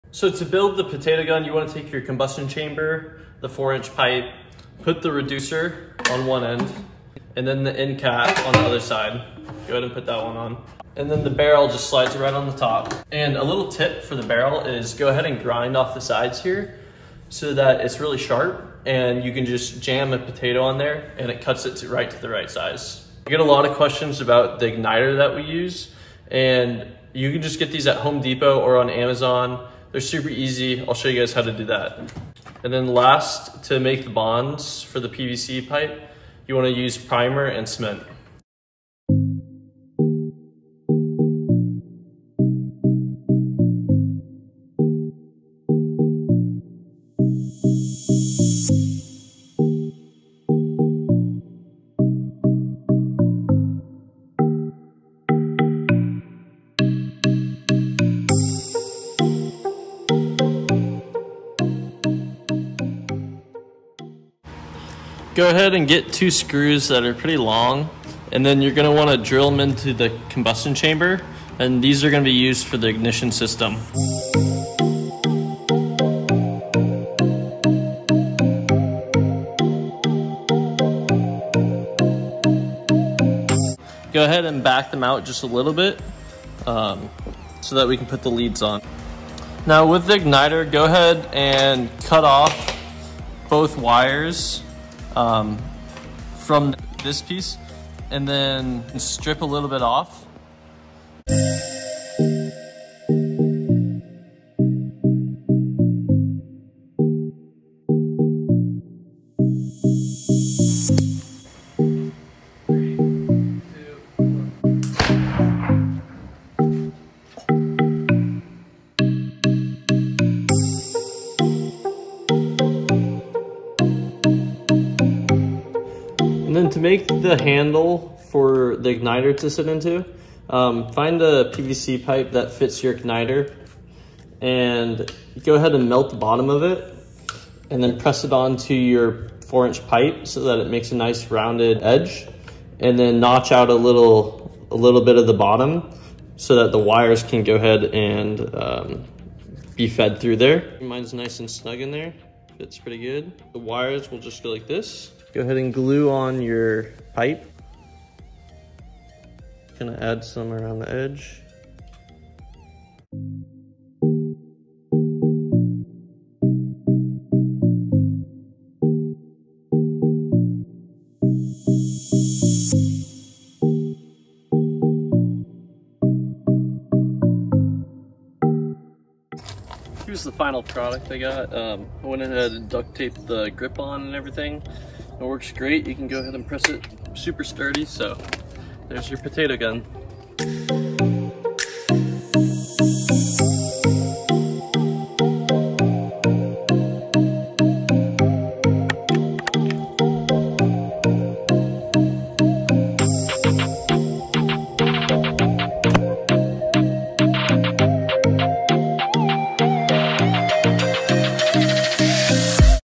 How to Build a Potato Gun DIY (Material In Description)
Igniter Kit
4” PVC pipe – 2 feet
2” PVC pipe – 4 feet
4” PVC threaded coupler
4” PVC cleanout cap
4” to 2” PVC adapter
PVC primer / cement

How to build a spud gun step by step at home.